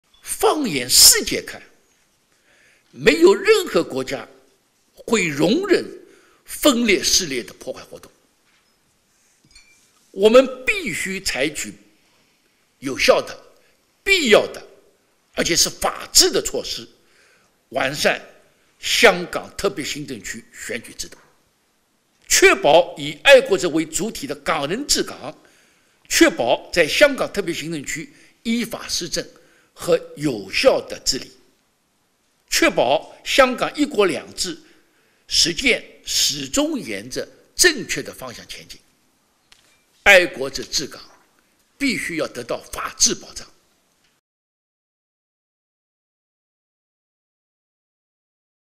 Han Zheng: 'Patriots governing Hong Kong' must be upheld under the rule of law
Chinese Vice Premier Han Zheng called for the principle of "patriots governing Hong Kong" to be upheld under the rule of law, as he joined a panel discussion with members from the Hong Kong and Macao delegation to the fourth session of the 13th National Committee of the Chinese People's Political Consultative Conference (CPPCC) on Saturday.